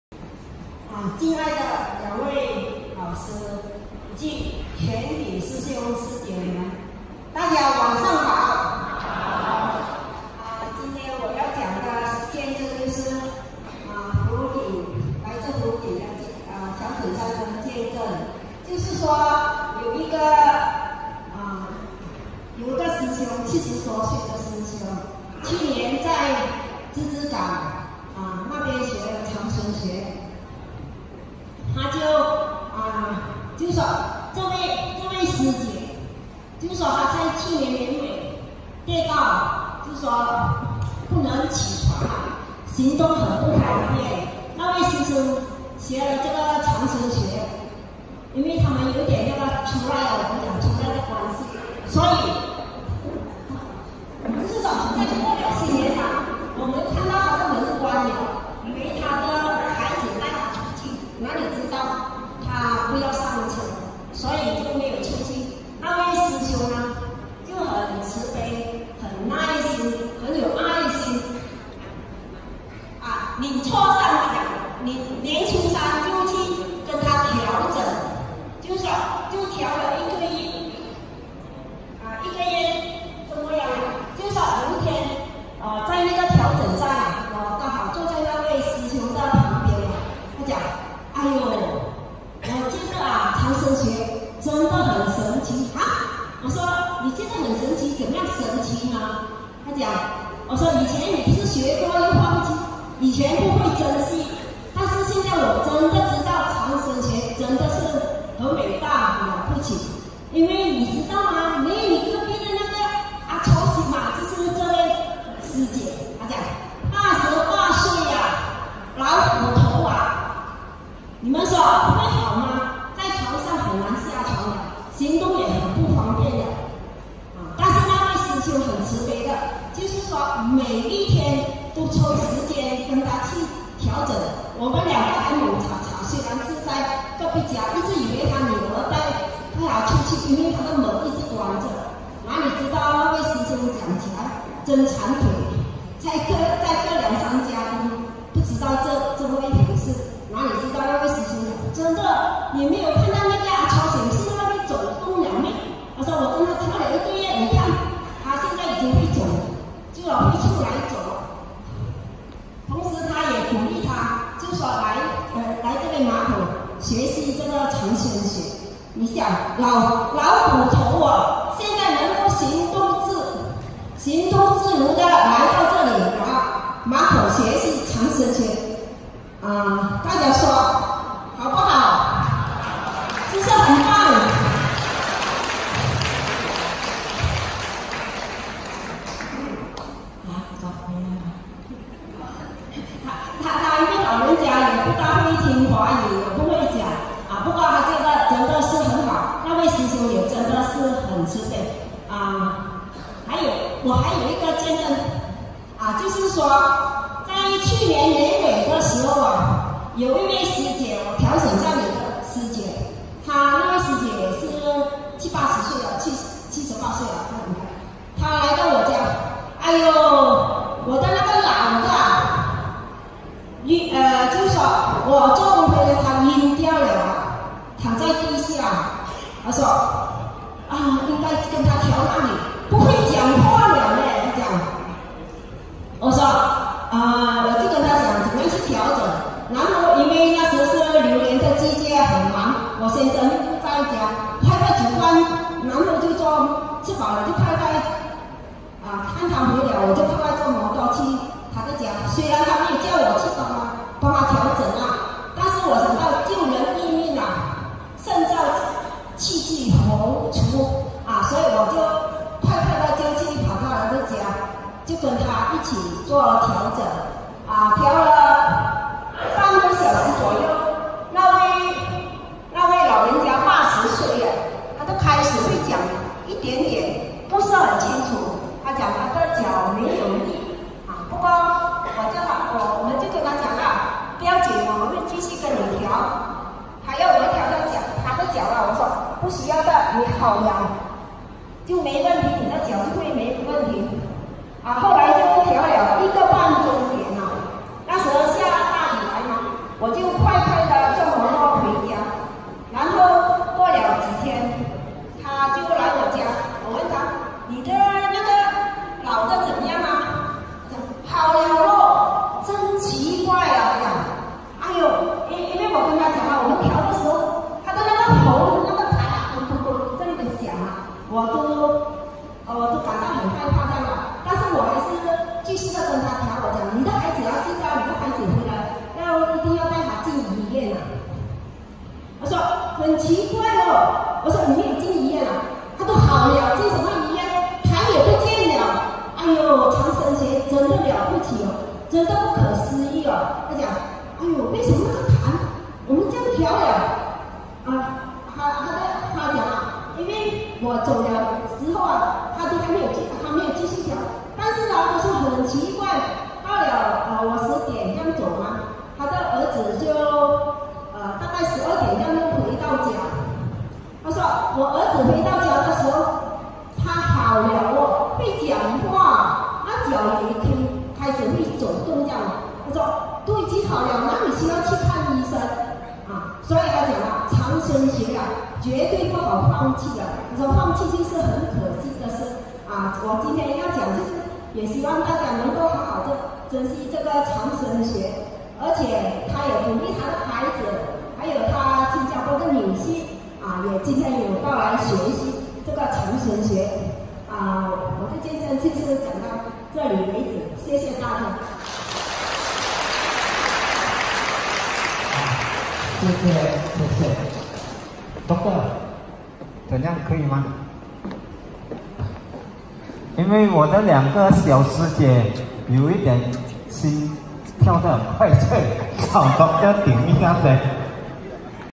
老人家摔倒.頭暈.痰梗住（長生學見證系列）Longevitology
她88歲摔倒以後臥病不起，還好長生學幫我調整一個月，讓我又可以起床走路到處跑！老人家頭暈、痰梗住的呼吸道⋯⋯
歐瑞英 師姐見證分享
2018年11月29日 馬來西亞 森美蘭 馬口